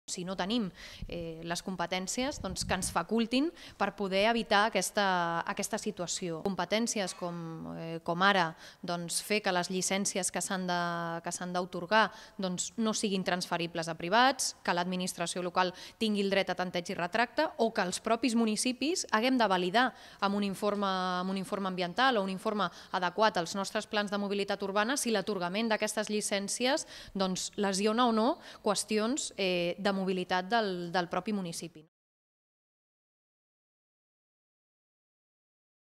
Mercedes Vidal sobre les llicències de taxis
L'Ajuntament de Barcelona exposarà avui davant el Ministeri de Foment la seva posició respecte a la concessió de llicències VTC, i li demanarà que exerceixi les seves competències per regular el sector o que faculti els ajuntaments per poder-ho fer.